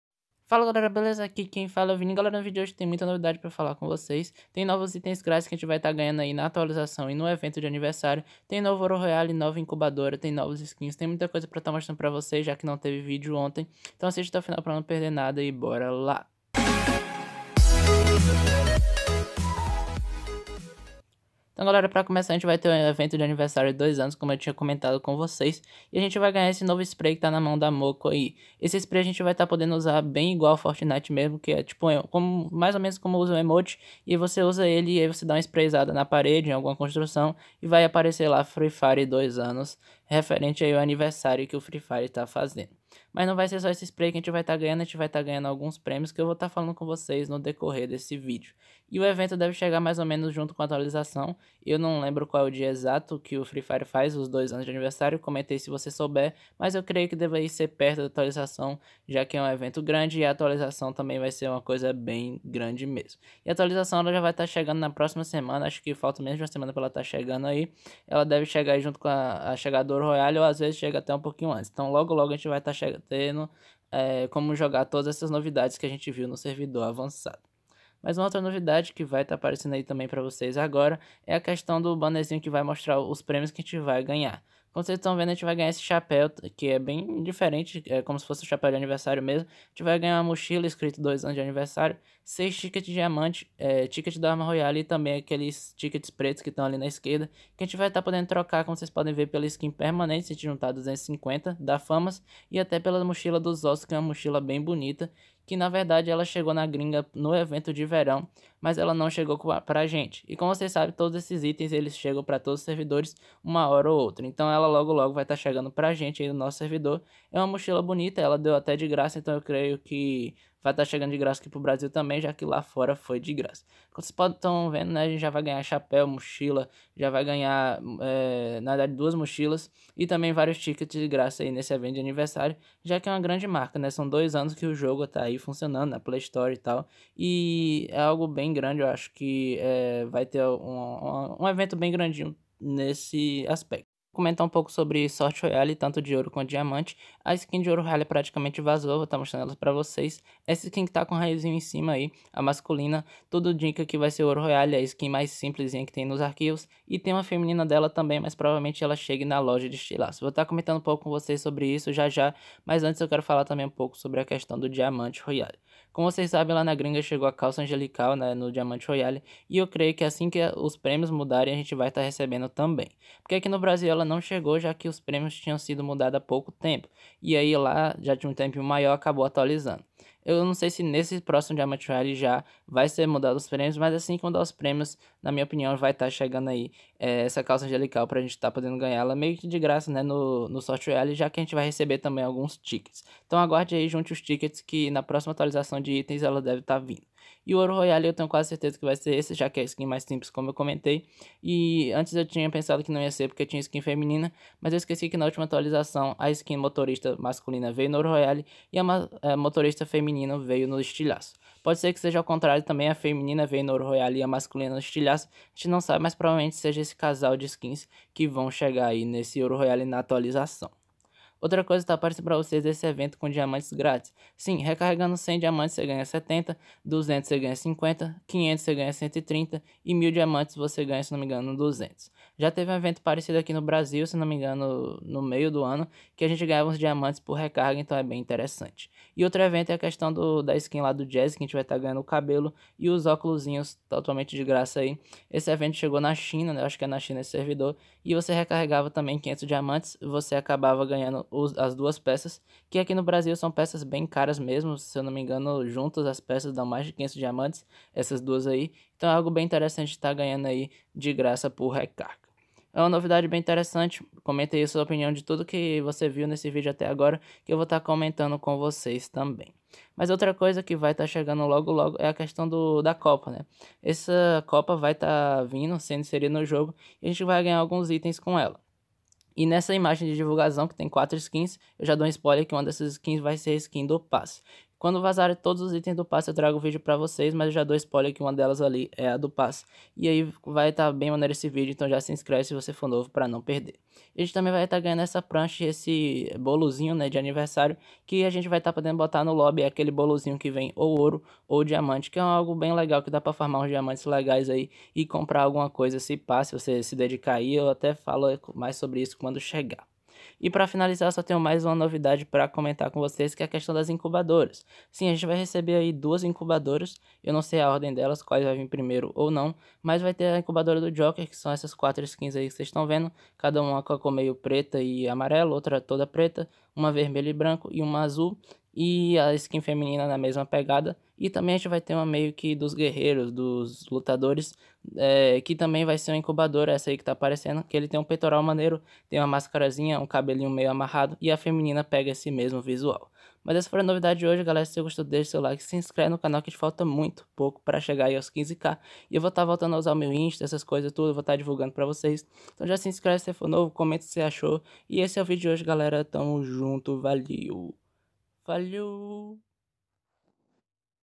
😱PRÓXIMO OURO ROYALE, DATA DA ATUALIZAÇÃO, ITENS GRÁTIS E CALÇA ANGELICAL - FREE FIRE
Sobre o vídeo de hoje temos muitas novidades sobre a atualização, como a sua data e também os eventos de 2 anos de aniversário do free fire que darão muitos itens grátis, a chegada da calça angelical no sorte royale, próximas incubadoras do free fire, próximo ouro royale do free fire e muito mais

Rede social

2018, nova atualização free fire, como ganhar